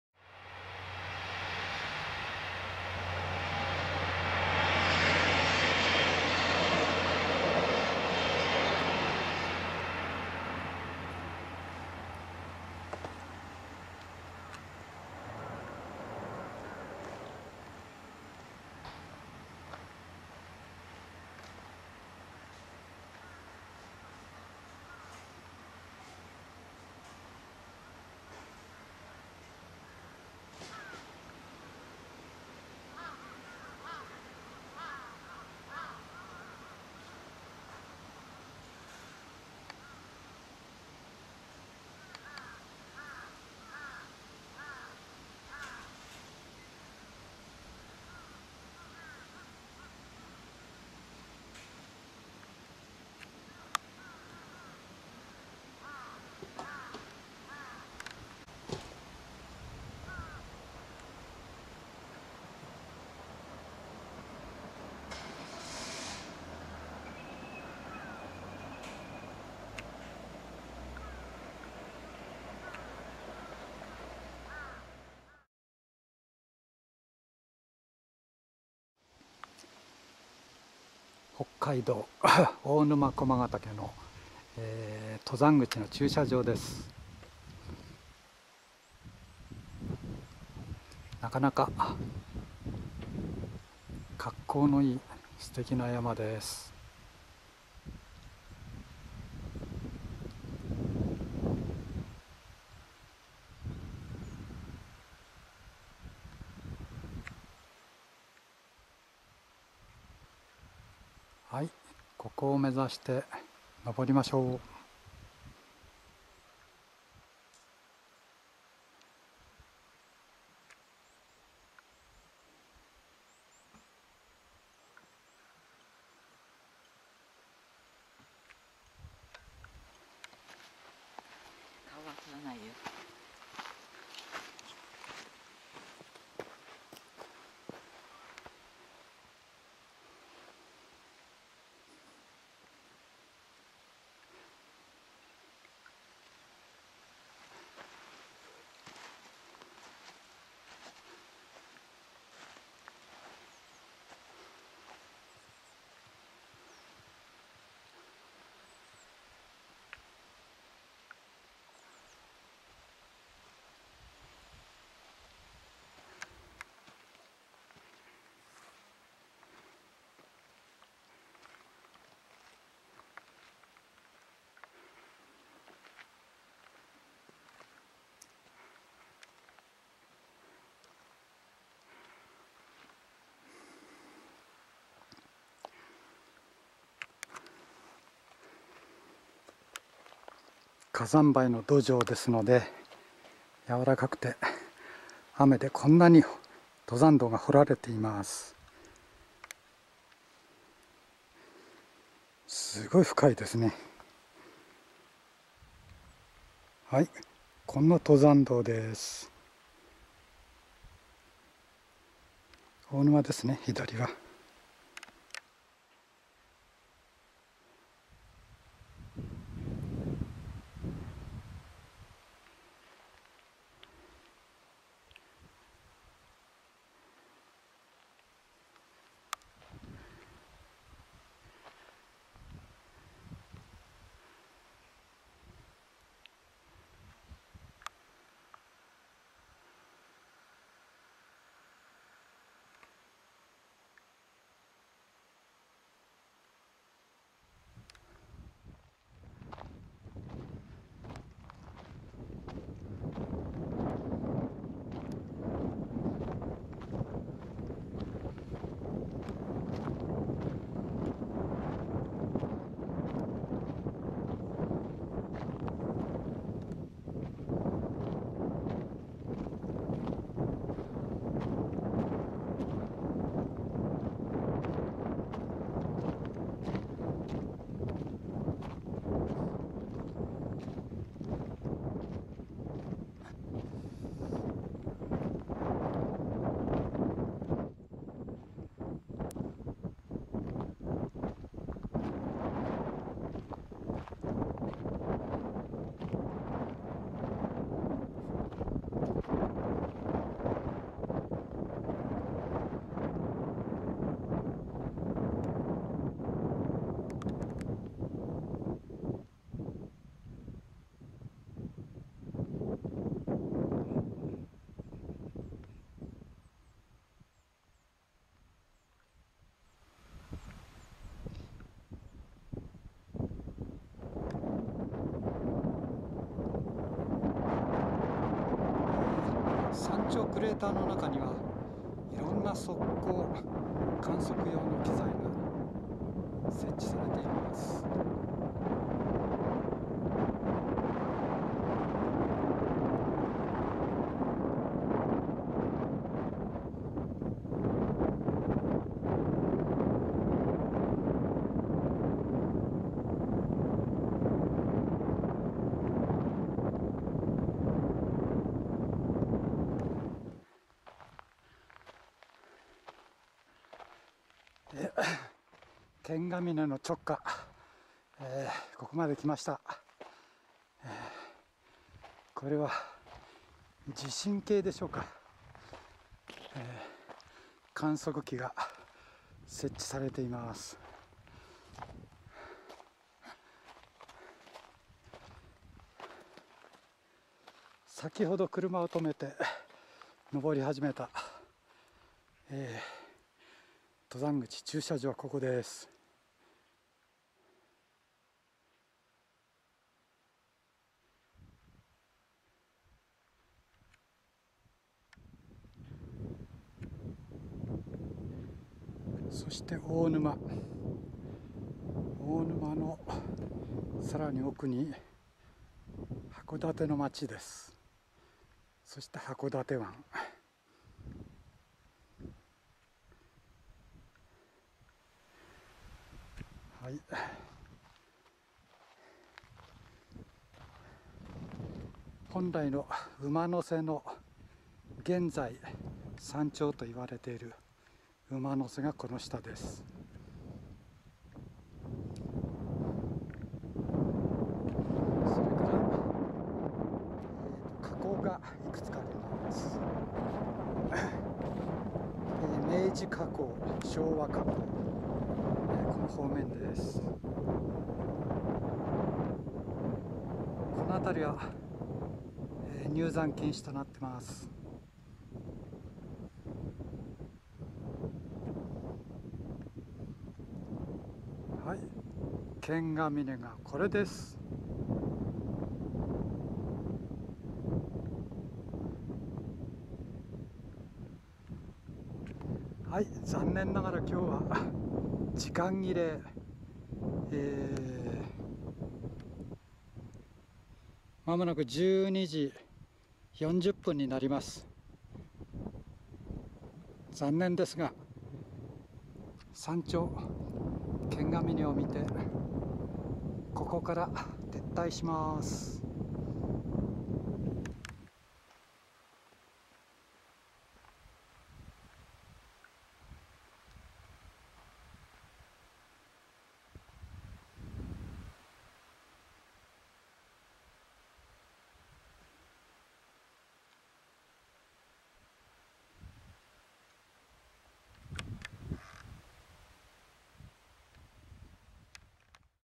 大沼公園から駒ケ岳2012
2012年9月24日から北海道函館を基点にして駒ケ岳と樽前山に行った。
TVニュースで倶知安付近で大雨強風竜巻災害で大型トラックが横転したり、JR特急が倒木と衝突する大変な天候であったが、スポット的に我々移動して行く処が昼間は好いお天気に恵まれるという珍しい現象だった。
yamaharuo